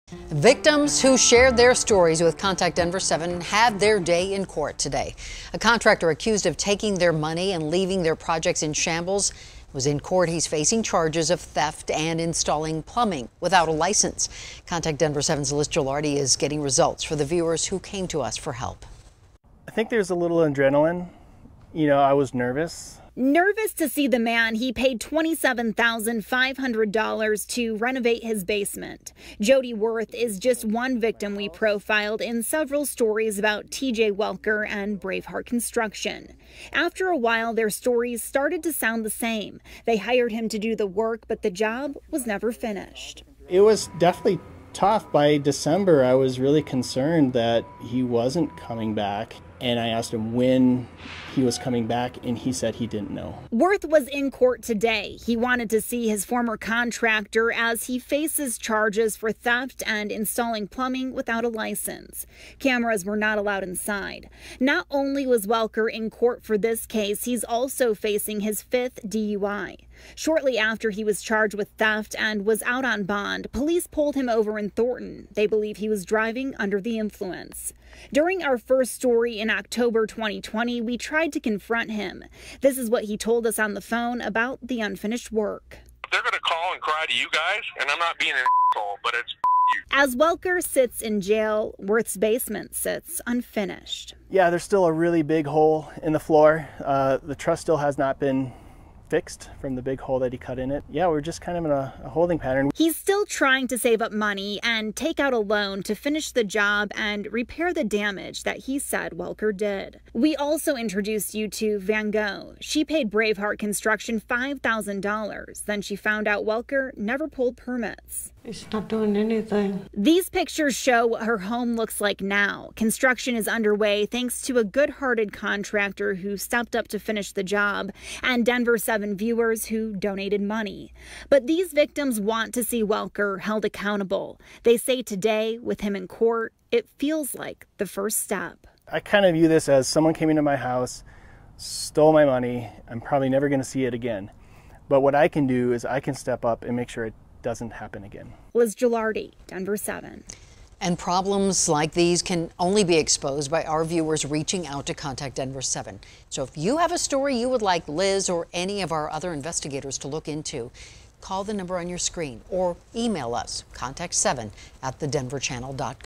Victims go to court to see contractor who they say took money, left work unfinished
Jody Wirth still has a hole in his basement floor where he says TJ Welker started renovations but never finished the job. Now, Welker is facing theft charges, and he appeared in court Thursday.